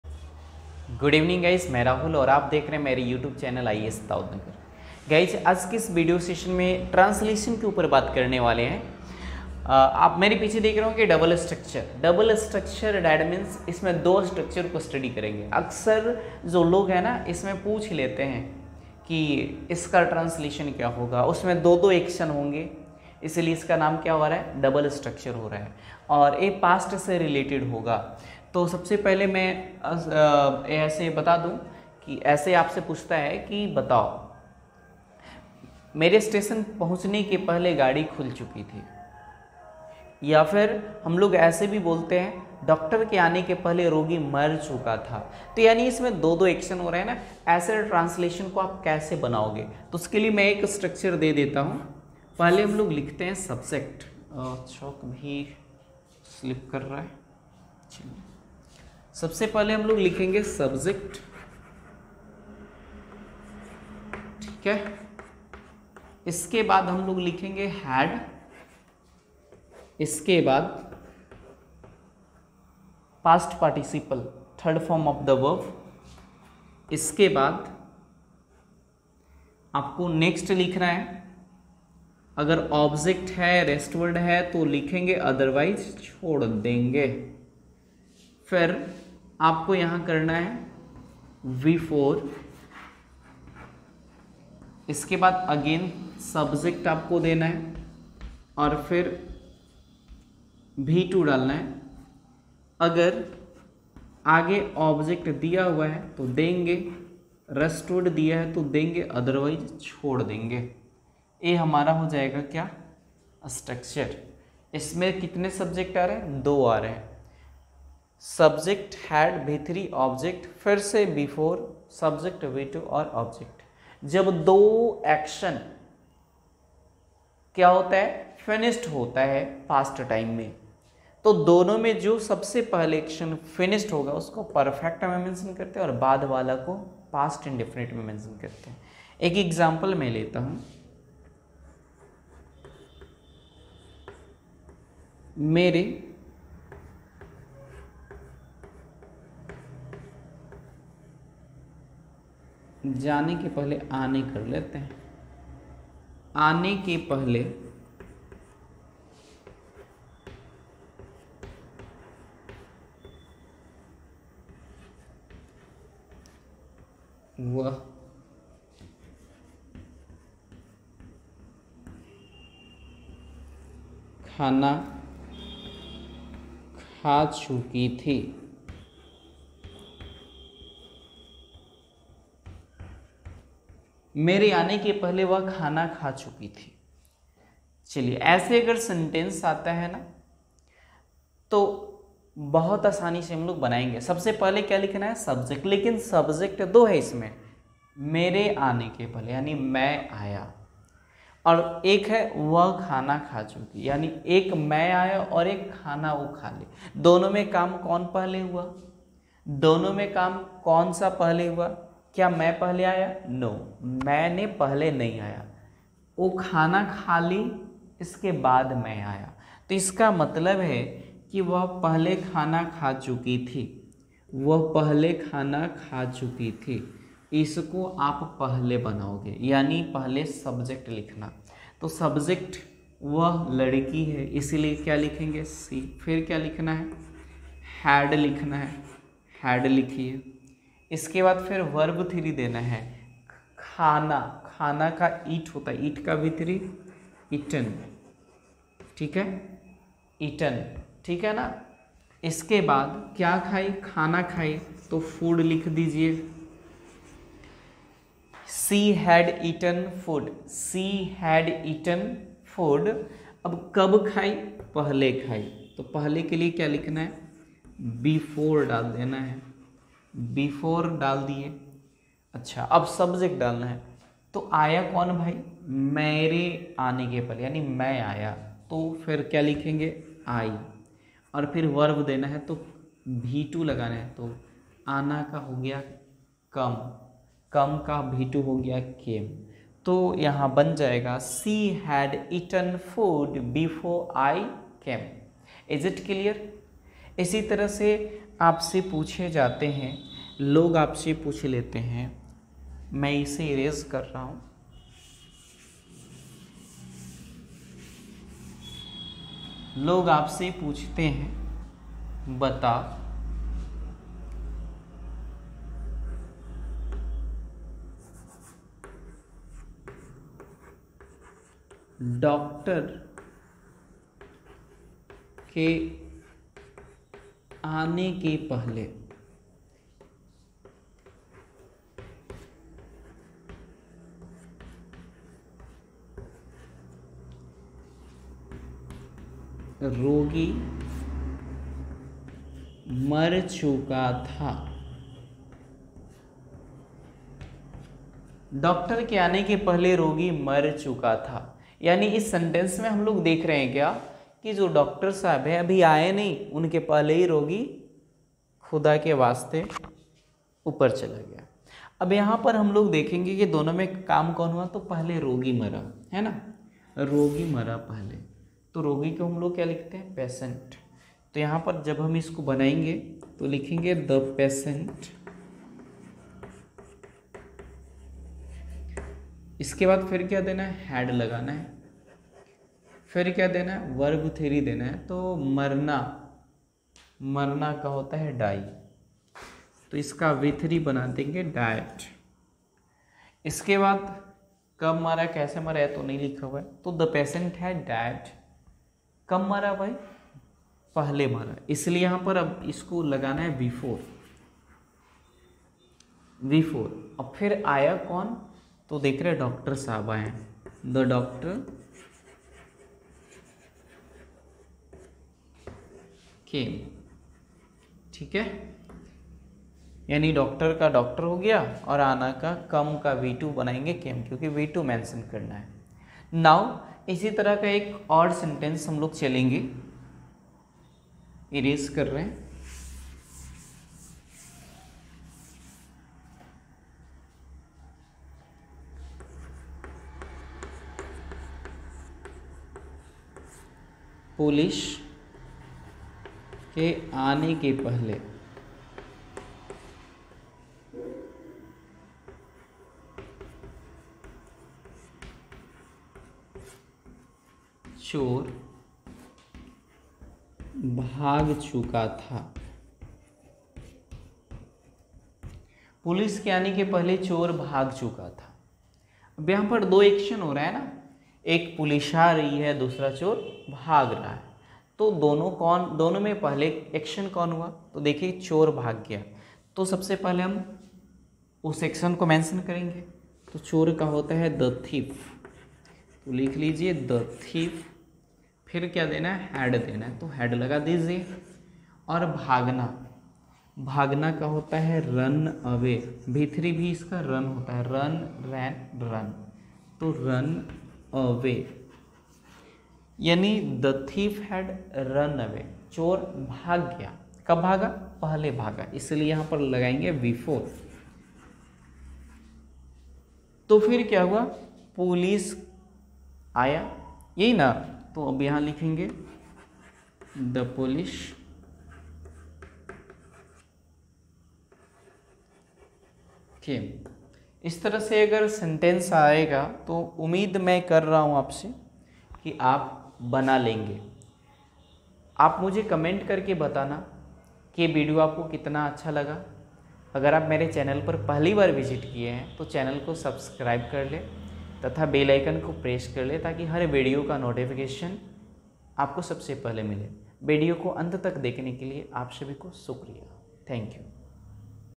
double Structure
how to translate

past perfect Vs Past Indefinite

pulish ke aane ke pahle chor bhag chukar tha